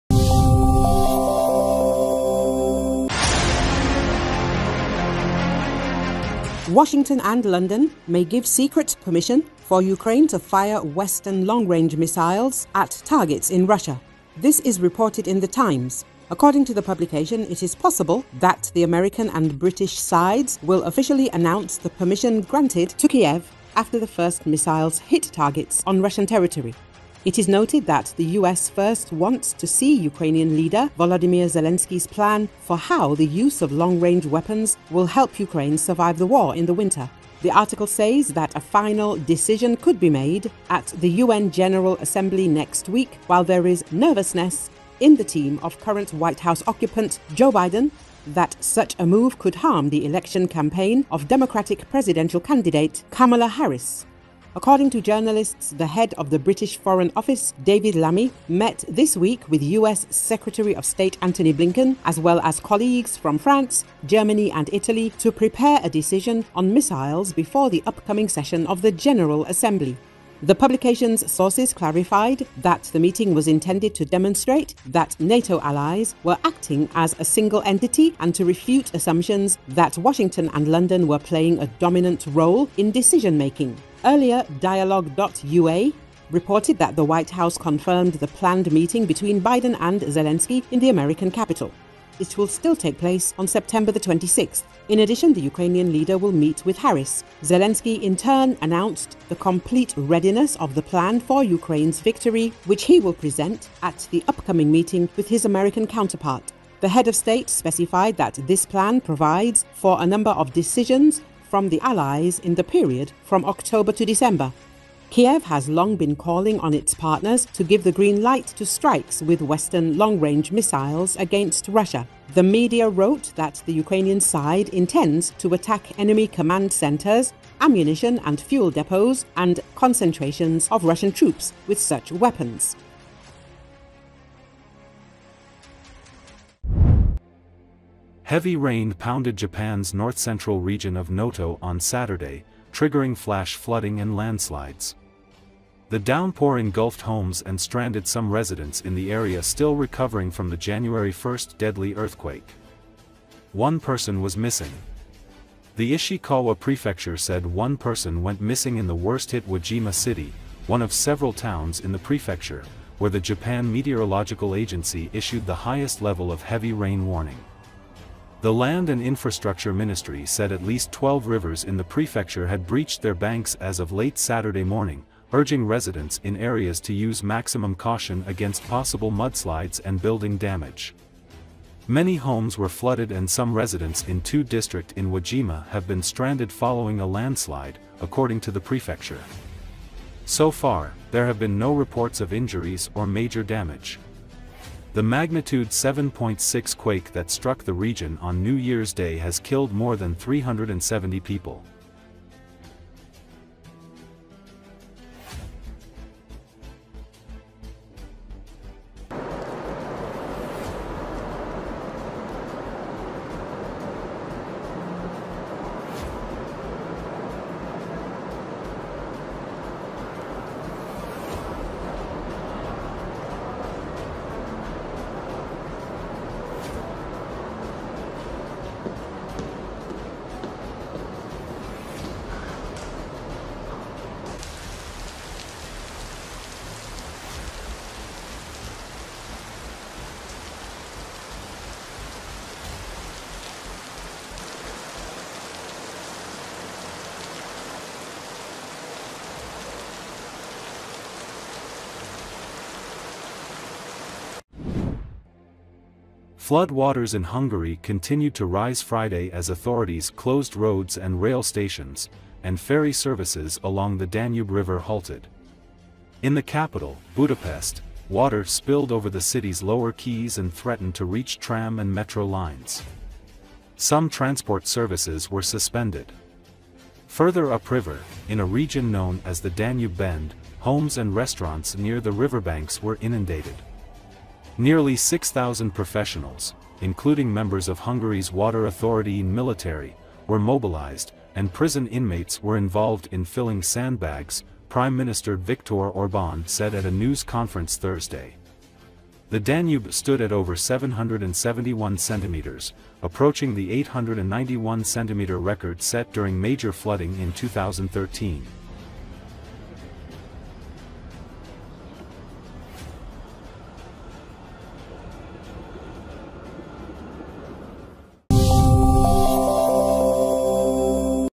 Ukraine to get secret permission to fire Western missiles deep into Russia
Washington and London may give secret permission for Ukraine to fire Western long-range missiles at targets in Russia.  This is reported in  The Times.

According to the publication, it is possible that the American and British sides will officially announce the permission granted to Kiev after the first missiles hit targets on Russian territory . It is noted that the US first wants to see Ukrainian leader Volodymyr Zelensky's plan for how the use of long-range weapons will help Ukraine survive the war in the winter.  The article says that a final decision could be made at the UN General Assembly next week, while there is "nervousness" in the team of current White House occupant Joe Biden that such a move could harm the election campaign of Democratic presidential candidate Kamala Harris.  According to journalists, the head of the British Foreign Office, David Lammy, met this week with US Secretary of State Antony Blinken, as well as colleagues from France, Germany and Italy, to prepare a decision on missiles before the upcoming session of the General Assembly.  The publication's sources clarified that the meeting was intended to demonstrate that NATO allies were acting as a single entity and to refute assumptions that Washington and London were playing a dominant role in decision-making.  Earlier,  Dialog.UA  reported that the White House  confirmed the planned meeting between Biden and Zelensky in the American capital. It will take place on September 26. In addition, the Ukrainian leader will meet with Harris.  Zelensky, in turn, announced the complete readiness  of the plan for Ukraine's victory, which he will present at the upcoming meeting with his American counterpart. The head of state specified that this plan provides for a number of  decisions from the allies in the period from October to December.  Kiev has long been calling on its partners to give the green light to strikes with Western long-range missiles against Russia. The media wrote that the Ukrainian side intends to  attack  enemy command centers, ammunition and fuel depots, and concentrations of Russian troops with such weapons.